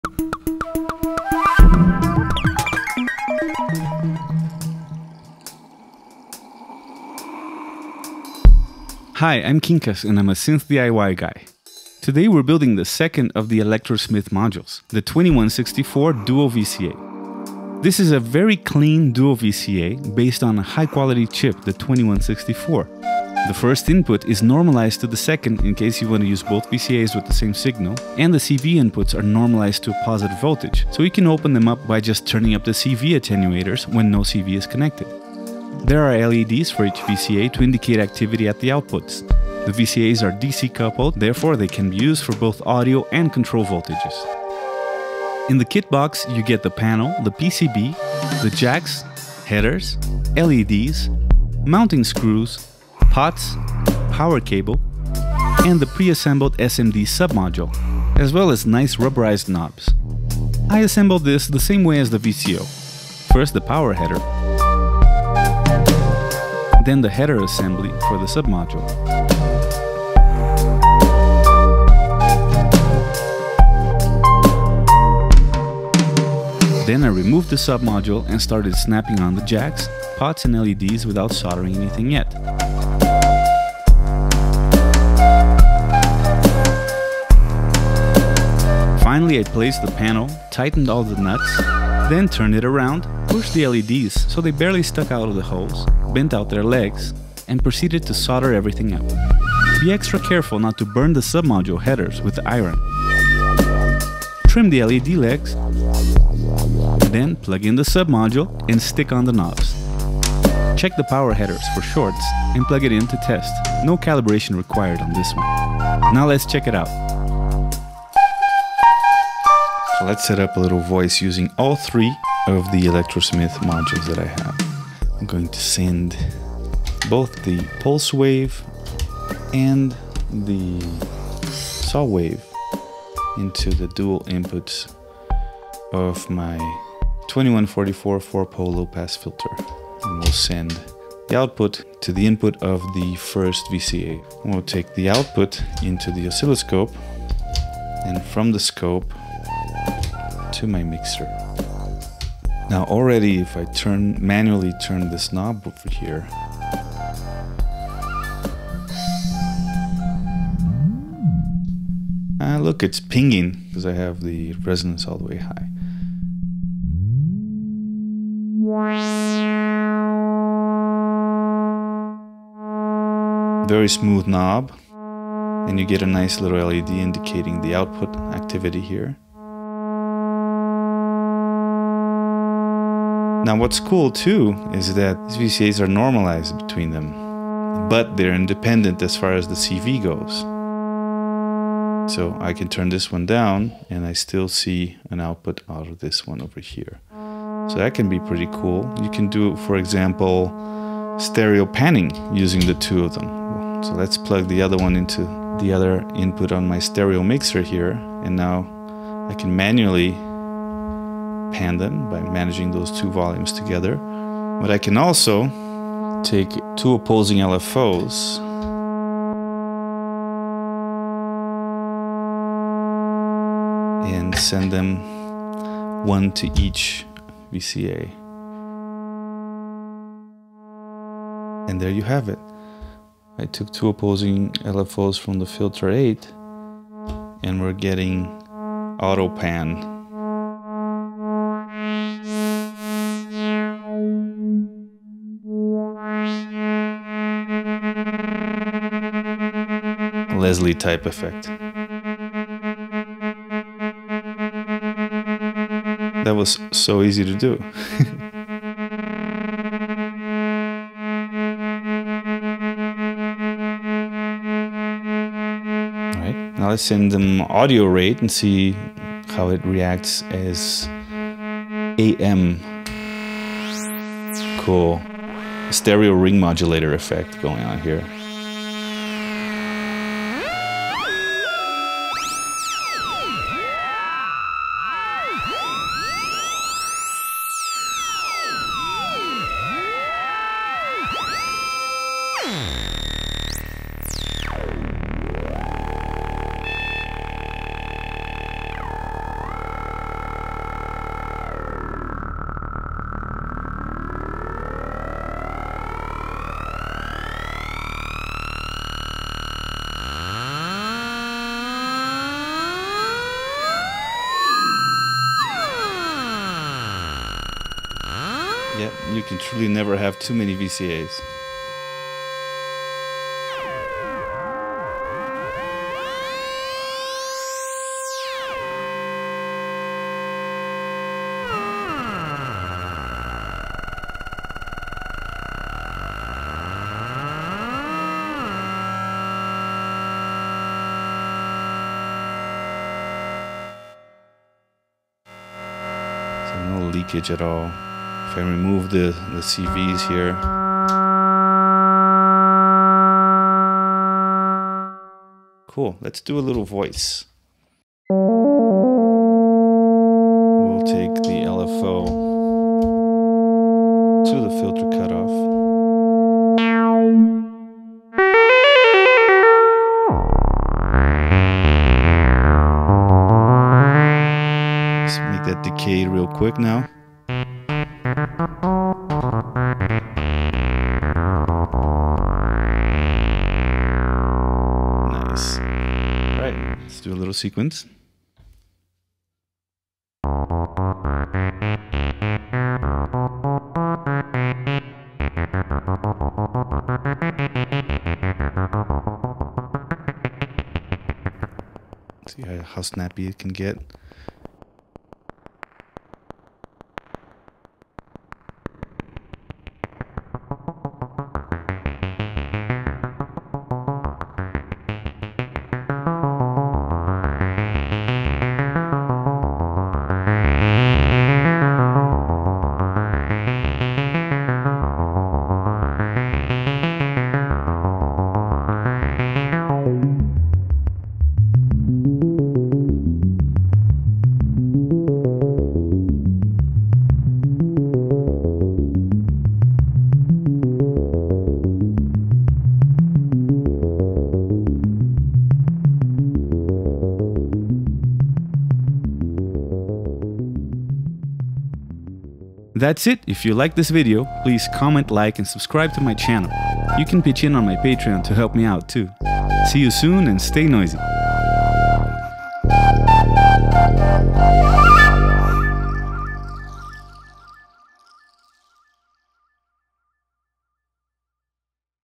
Electro-Smith 2164 Dual VCA - Kit Overview
Now it's time for the VCA module from the trio at Electro Smith! Another super easy, super high quality kit!

Video shot and edited by Quincas Moreira and Edgar López

Stay Noisy!